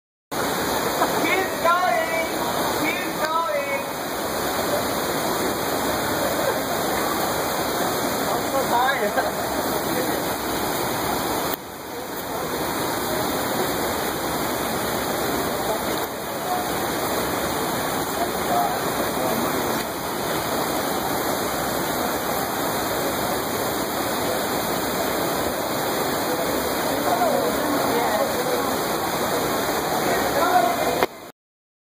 Watkins Glen Falls C 7 5 2014
This falls at Watkins Glen slips on down the sides of the gorge passing over the visitors below.